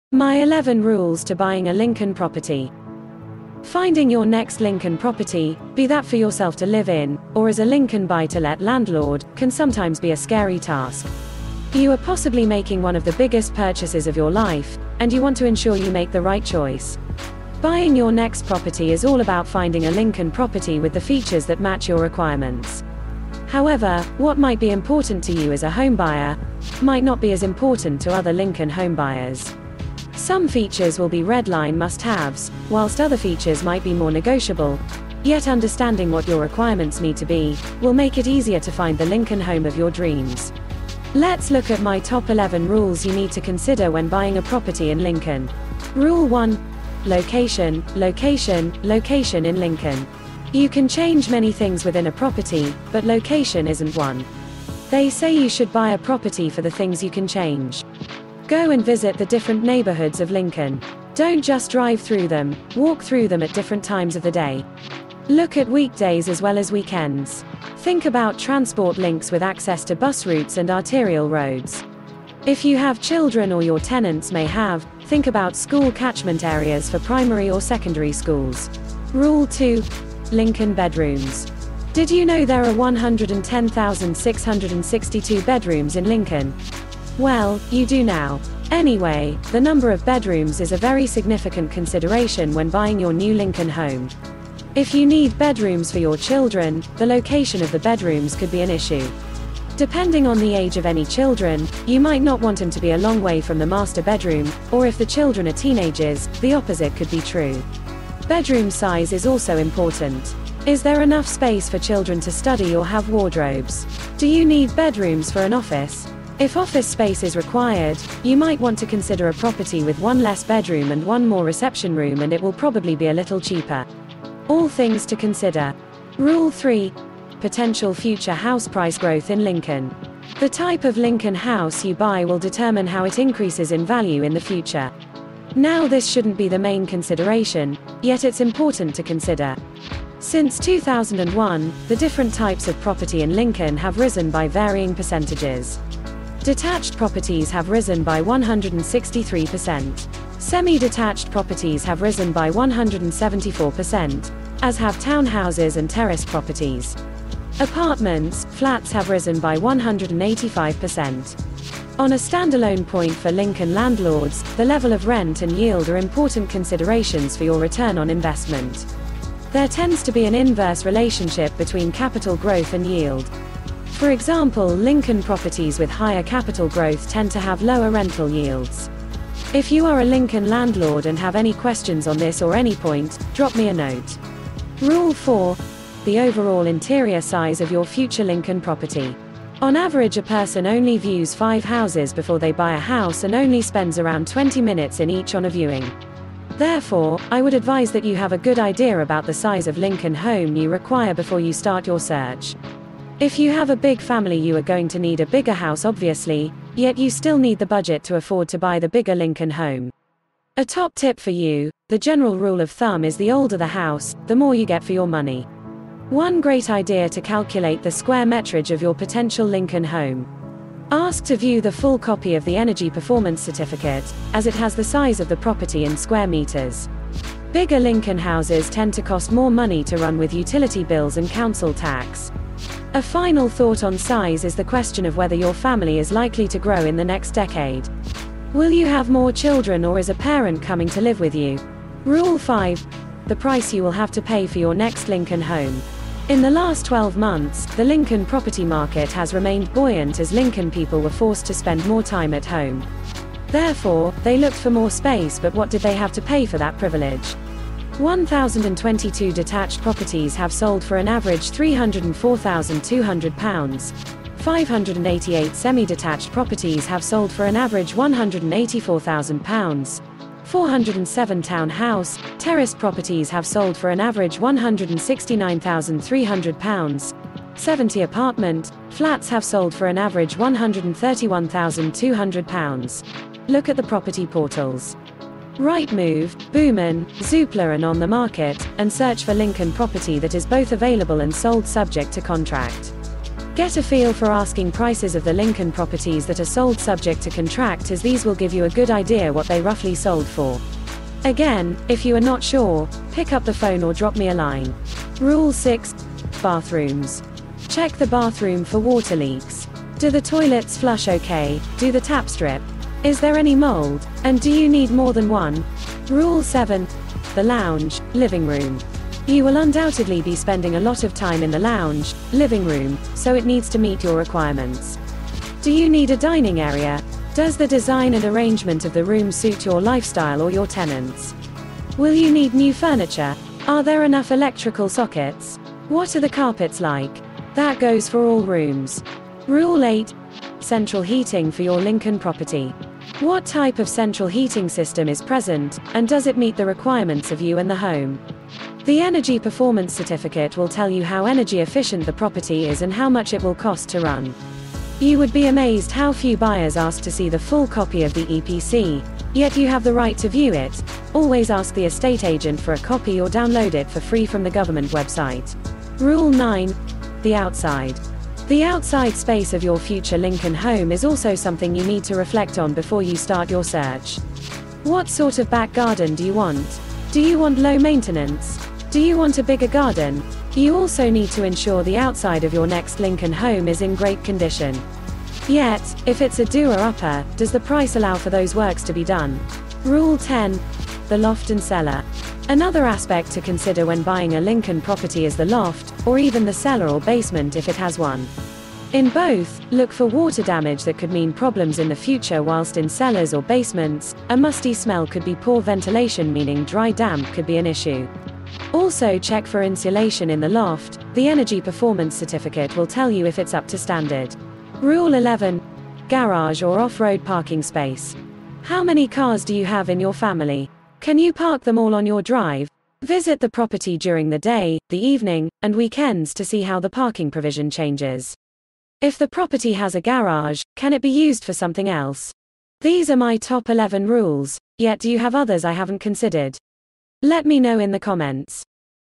My 11 Rules to Buying a Lincoln Property
Live in Lincoln and thinking of buying a property in 2022?

Buying your next property is all about finding a Lincoln property with the features that match your requirements. However, what might be important to you as a homebuyer, might not be as important to other Lincoln homebuyers or tenants.

These are my 11 rules that could help you make the right decision.